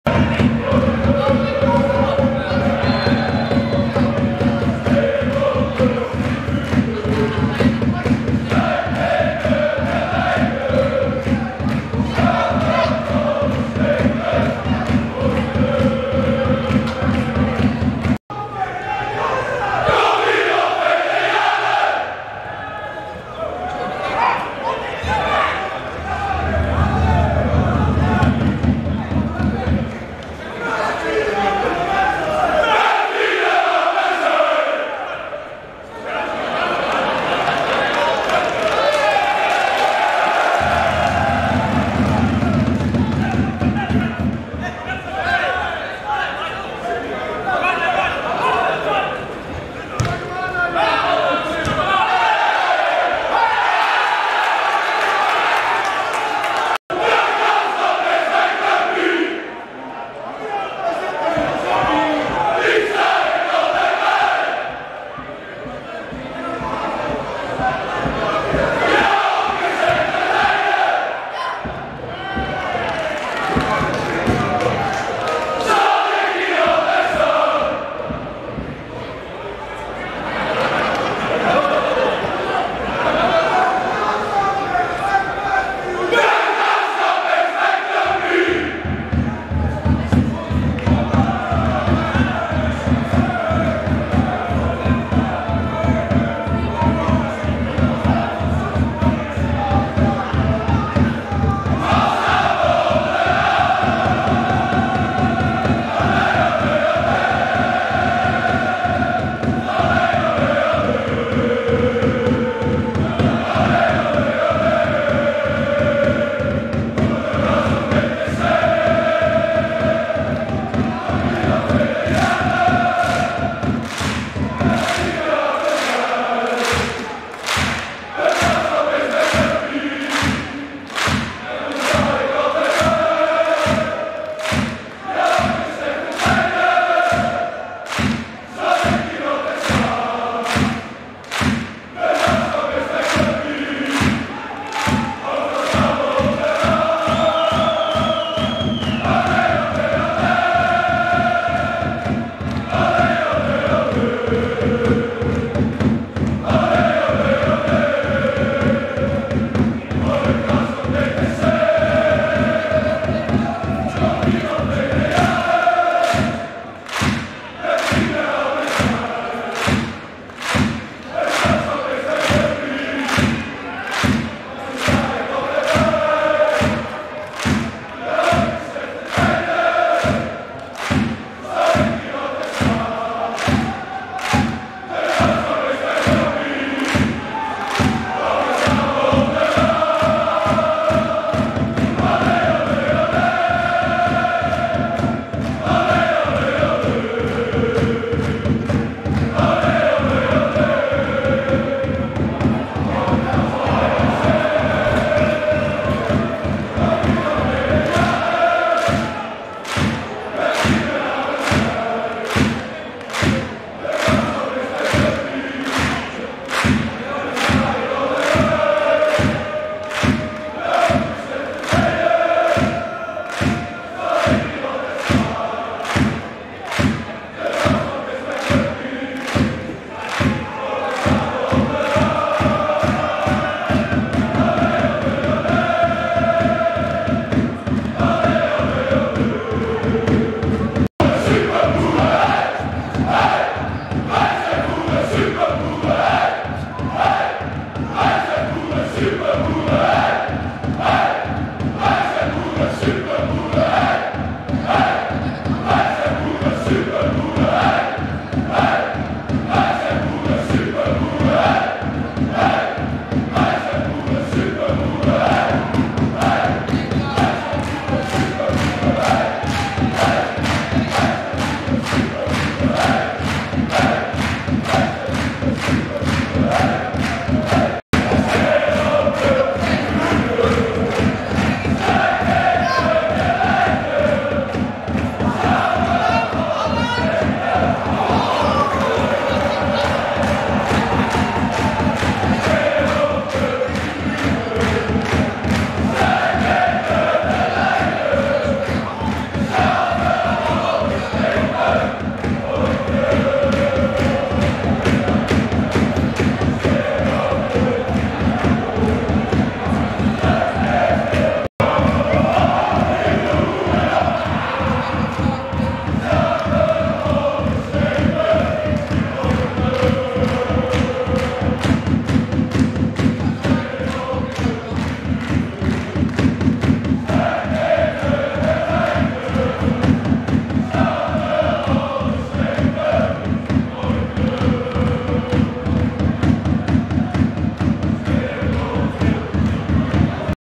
RKC - De Graafschap away support Brigata Tifosi
02/09/2017 - Away support van Brigata Tifosi bij de Jupiler League-wedstrijd tussen RKC Waalwijk en De Graafschap.